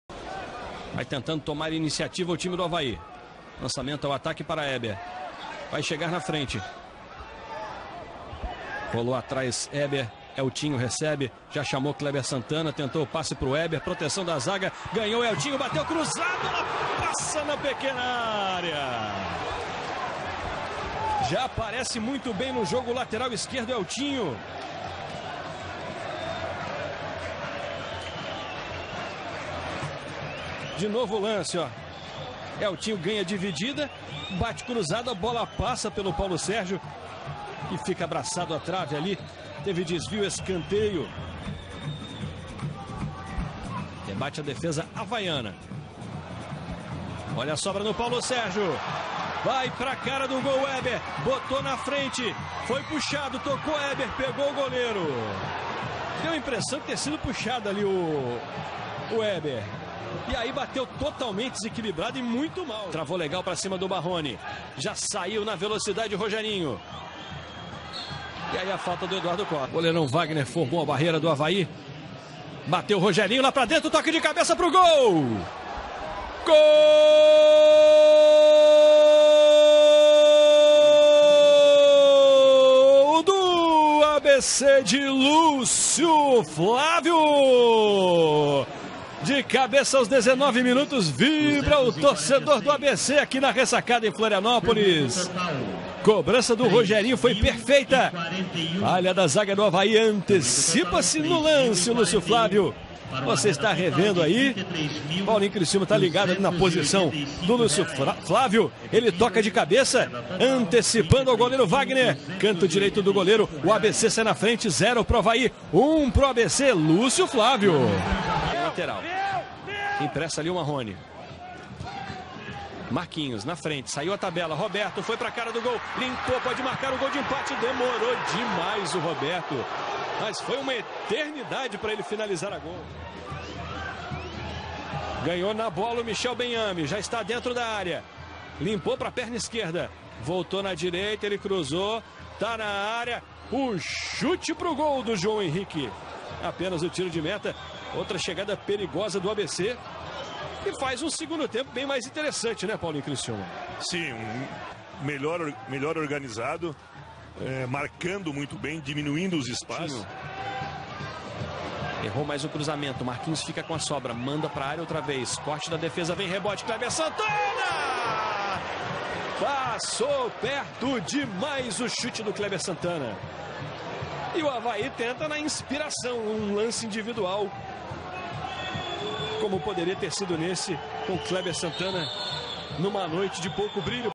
9ª rodada - Avaí 0 x 1 ABC
Assista os melhores momentos de Avaí/SC x ABC em partida válida pela 9ª rodada do Brasileirão.